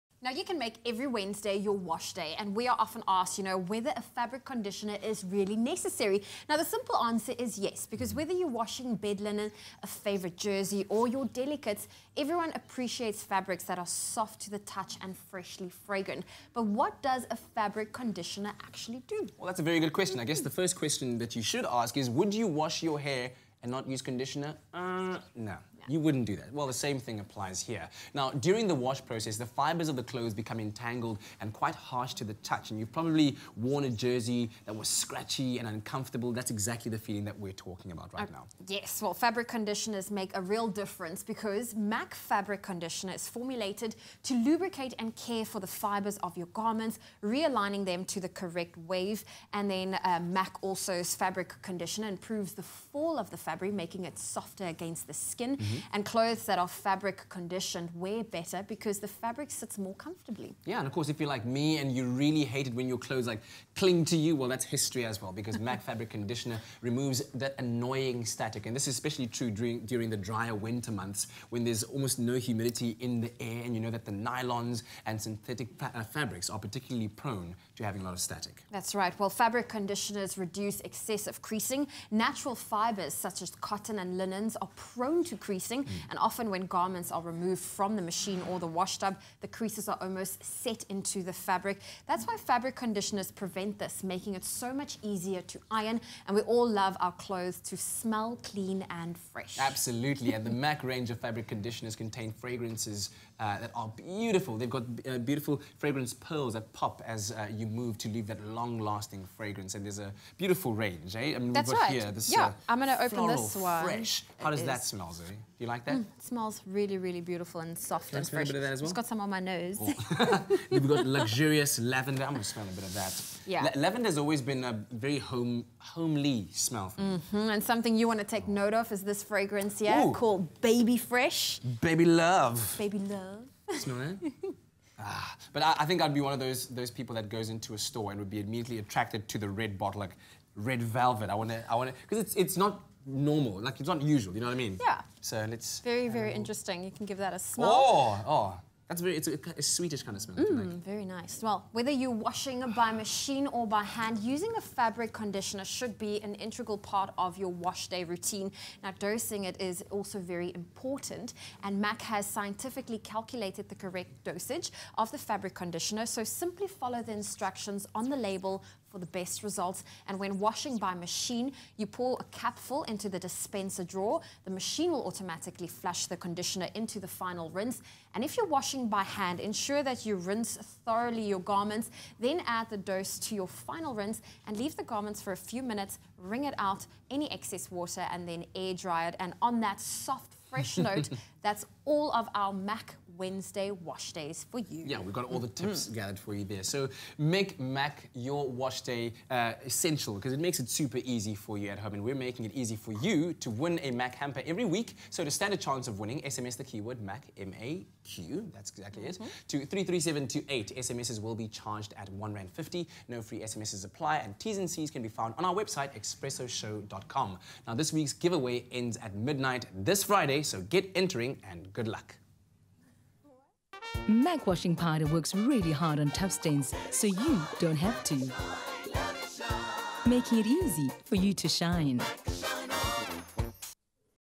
WEDNESDAY WASHDAY WITH MAQ – All About MAQ Fabric Conditioner
Make every Wednesday your wash day. We are often asked whether a fabric conditioner is really necessary.  The simple answer is yes. Whether you’re washing bed linen, a favourite jersey or your delicate, everyone appreciates fabrics that are soft to the touch and freshly fragrant.